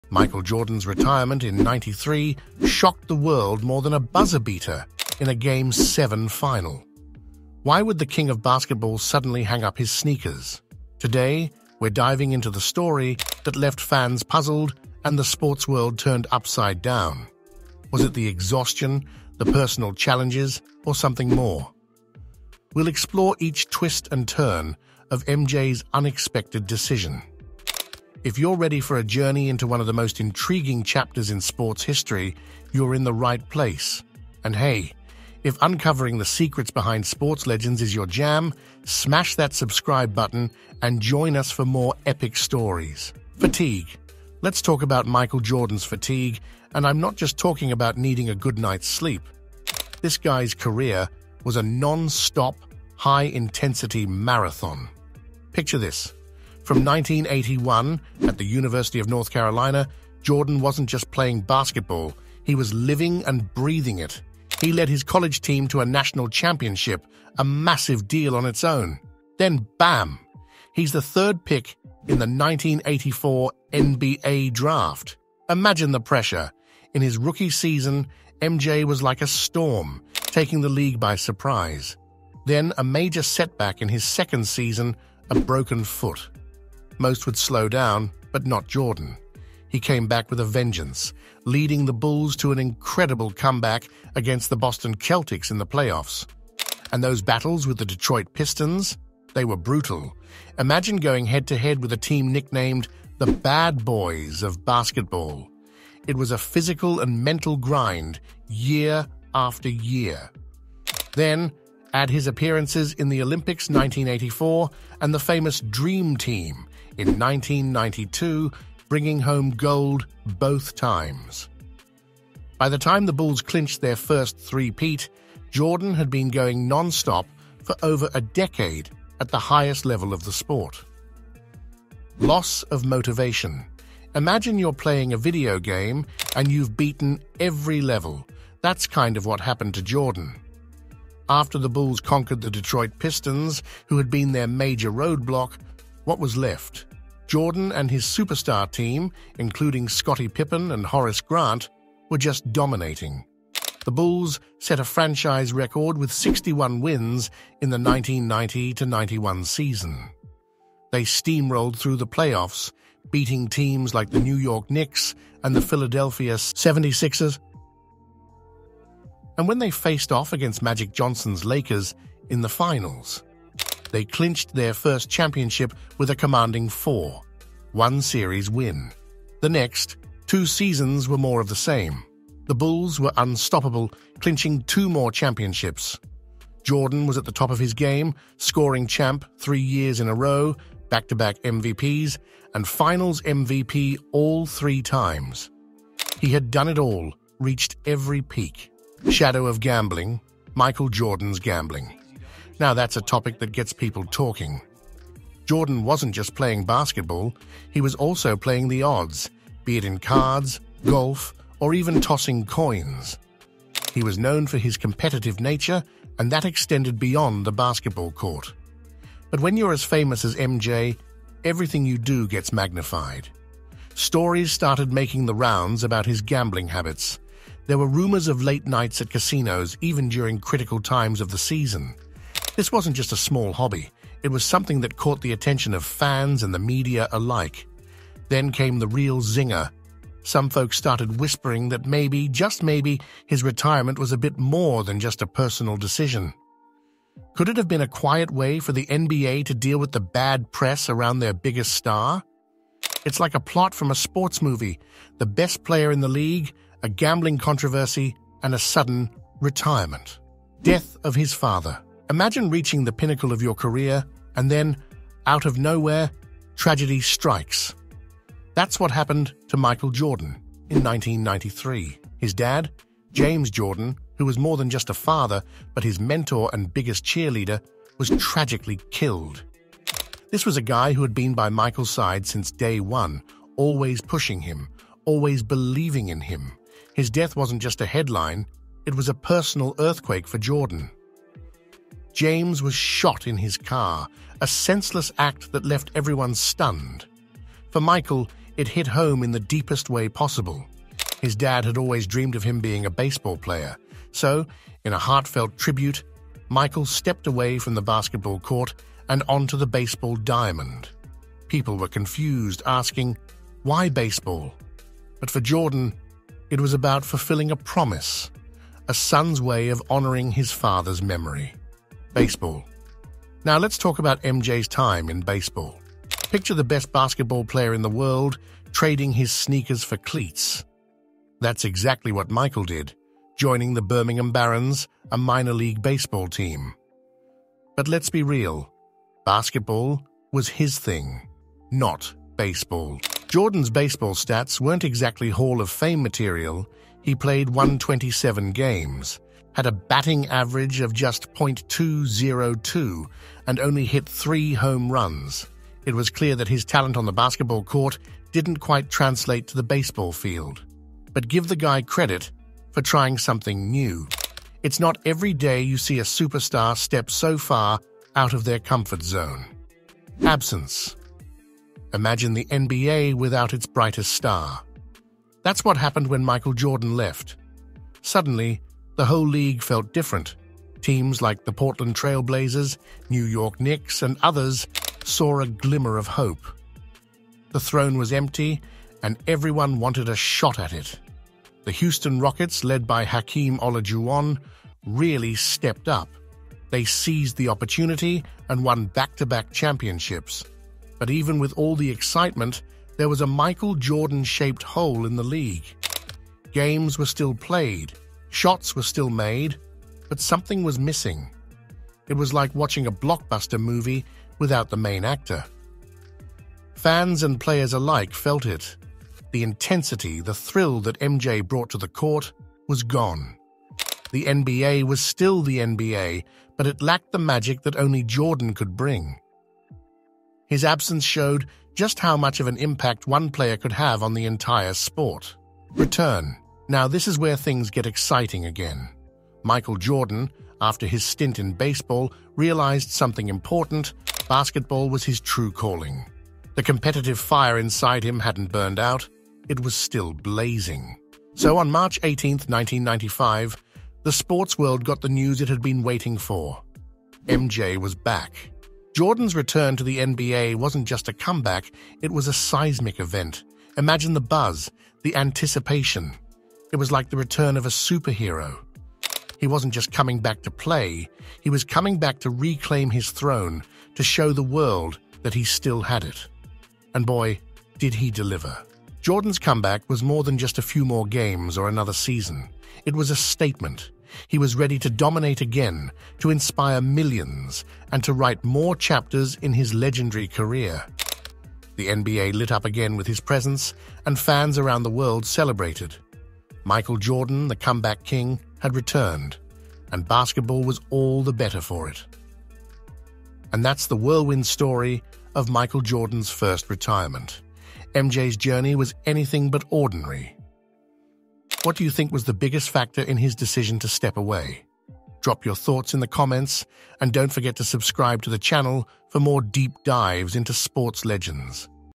The Hidden Truth: Michael Jordan’s Shocking First Retirement
In the midst of basketball's grandeur, the narrative of Michael Jordan's first retirement remains a captivating enigma, shrouded in mystery and speculation. This deep dive into the archives unveils the untold story behind his pivotal departure from the NBA, shedding light on the intricacies that fueled this significant chapter in sports history. Join us as we navigate through the shadows and decode the factors that led the basketball legend to take a leap of faith away from the court. This exploration goes beyond the surface, providing a unique perspective on the saga that defined a crucial juncture in Michael Jordan's iconic career.